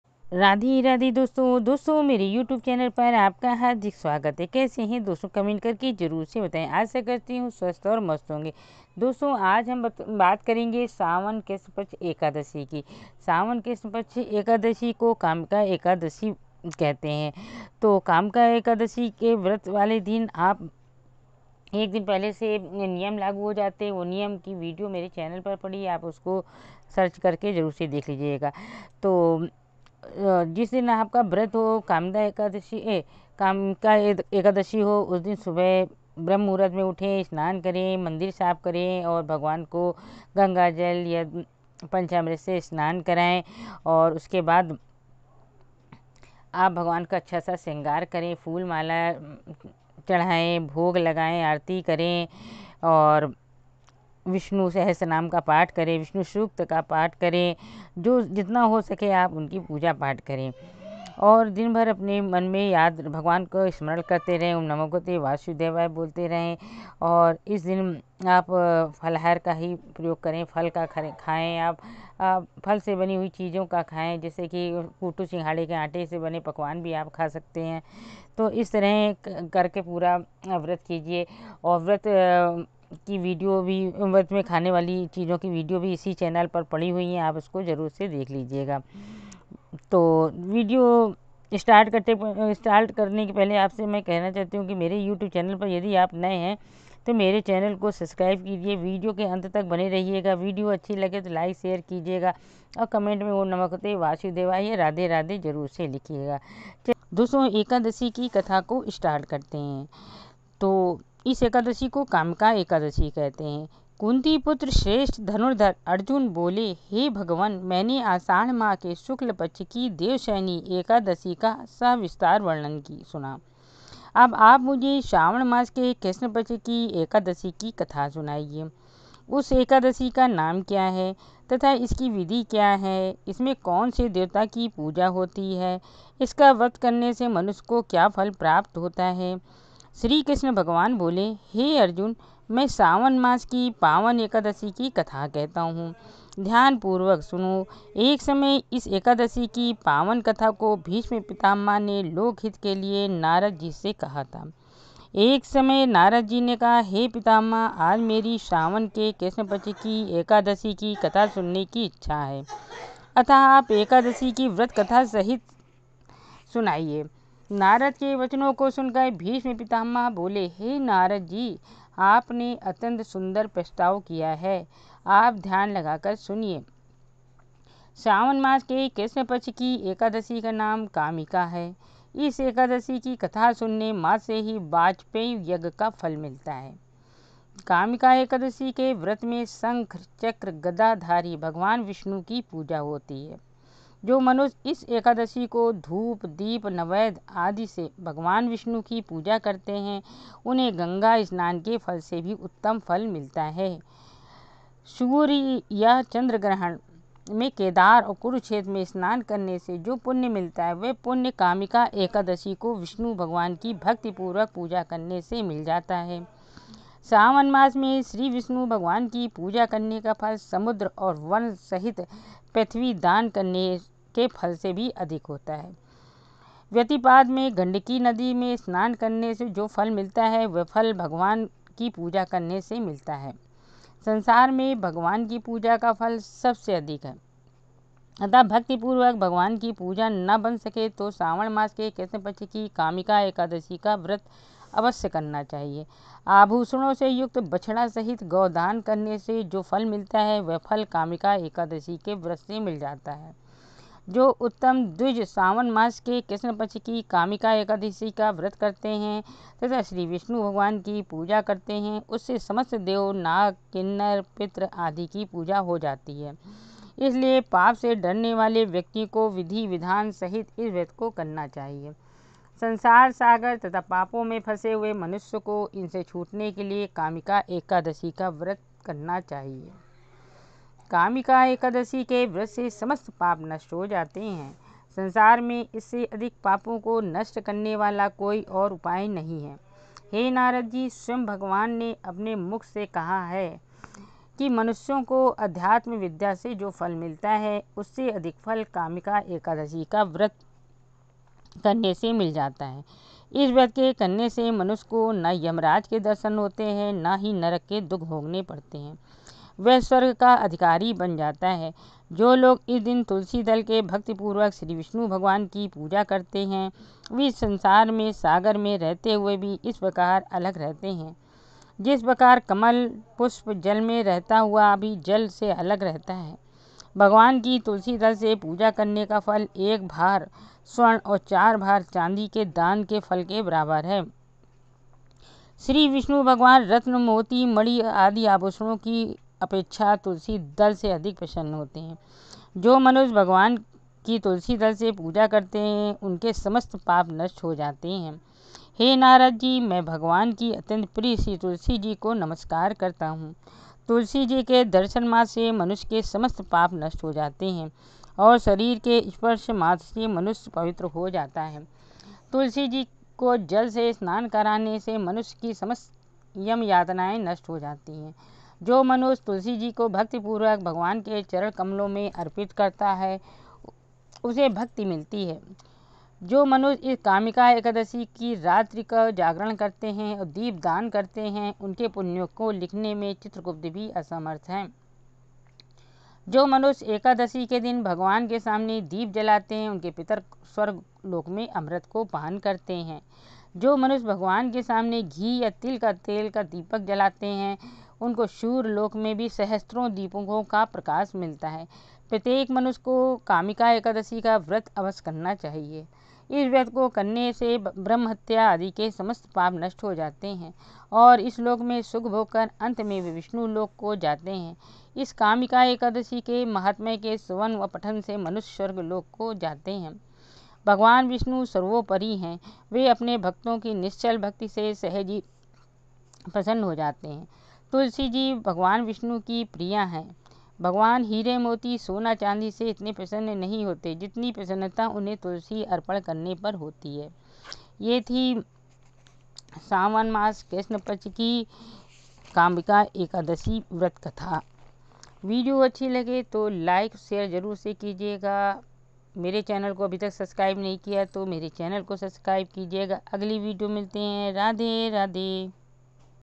श्रावण कृष्ण पक्ष एकादशी। कामिका एकादशी व्रत कथा। savan Krishna paksh kamika ekadashi vrat katha.
savan Krishna paksh kamika ekadashi vrat katha.
man Ambe Rasoi

राधे राधे
दोस्तों आज मैंने सावन कृष्ण पक्ष कामिका एकादशी व्रत कथा की वीडियो बनाई है। अधिक मास आने वाला है अधिक मास की कथा की वीडियो में डाल चुकी हूं उसको जरूर से देखिएगा अधिक मास और कथा में आगे डालती रहूंगी इसलिए मेरे चैनल को सब्सक्राइब करके रखिएगा वीडियो अच्छी लगी तो शेयर जरूर कीजिएगा

धन्यवाद

Maa Ambe Rasoi